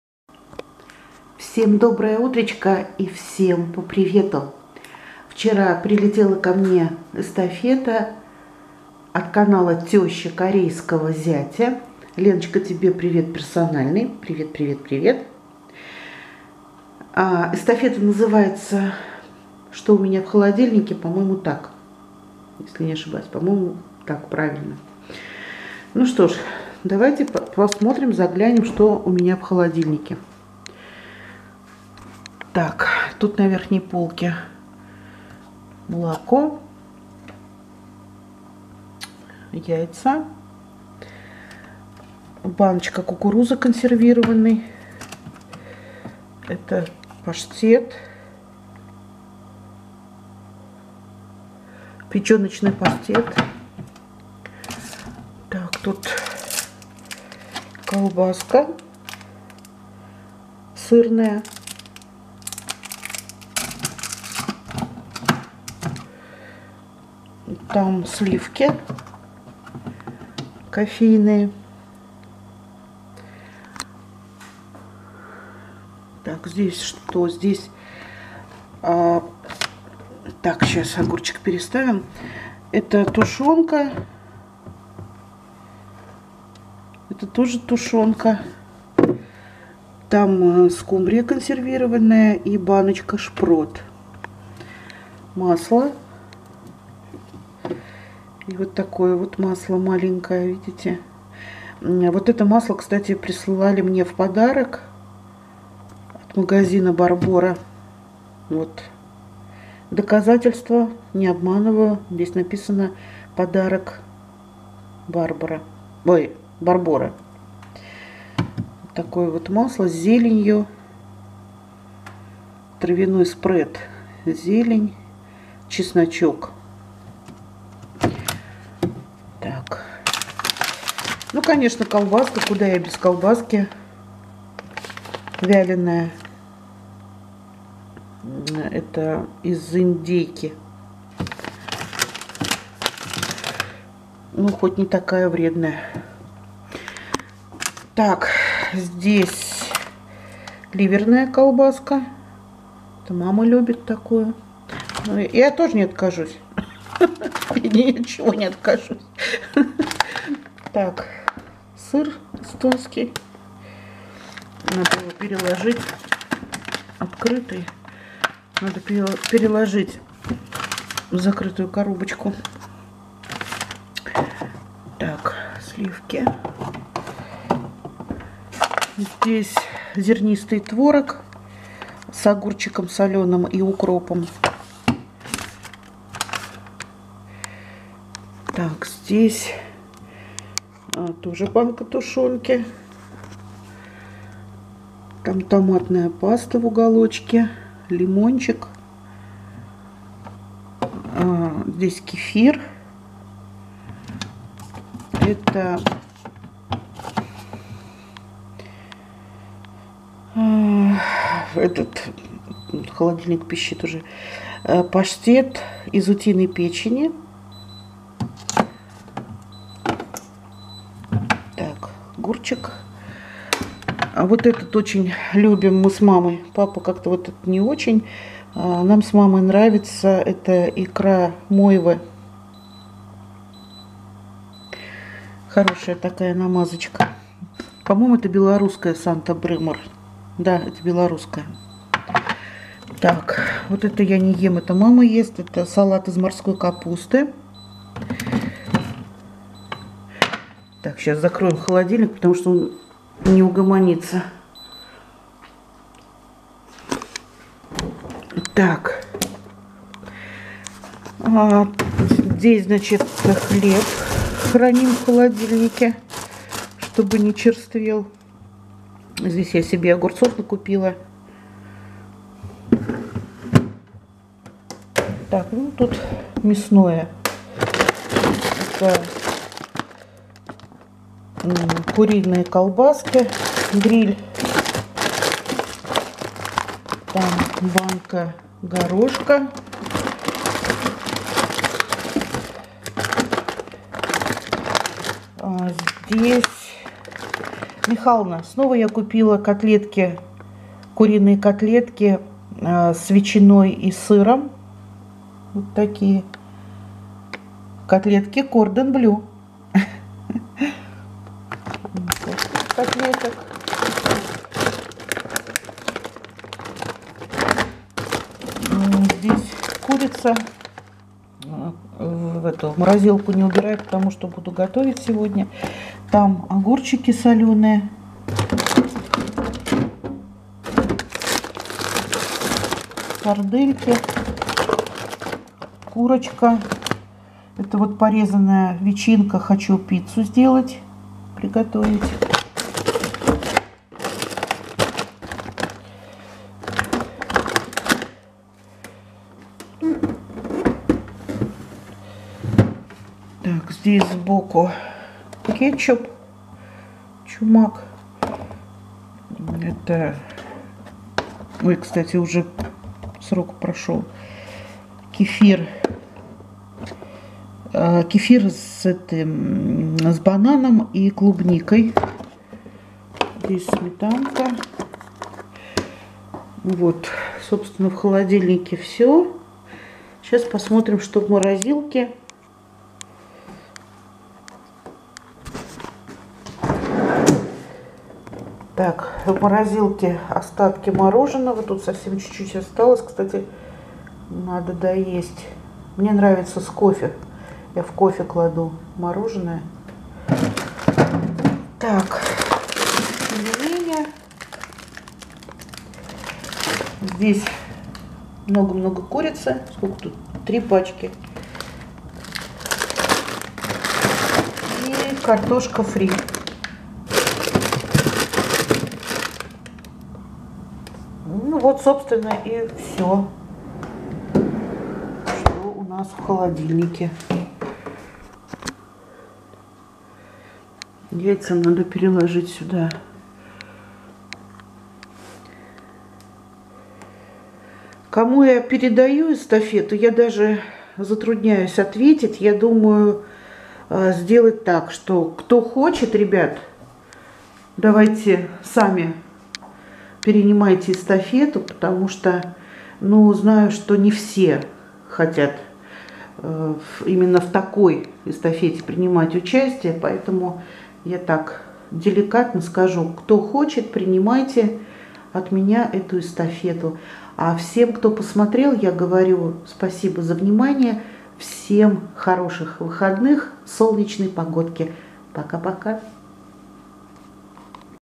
Эстафета/Какие продукты в холодильнике на время самоизоляции.
Эстафету получила от канала Теща Корейского Зятя

Дружу с Леночкой очень давно, практически с момента основания ее и моего канала. Заходите, не пожалеете!